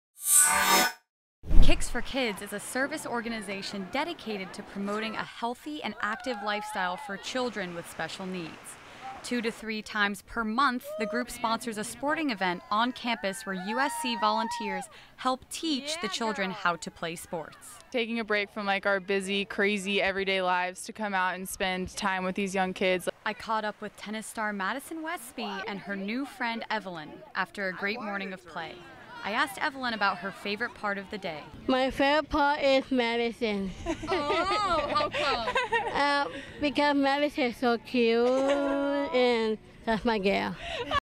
USC Tennis Player Volunteers For Kicks For Kids
USC Tennis star, Madison Westby takes her Sunday off to volunteer at USC's Kicks for Kids event.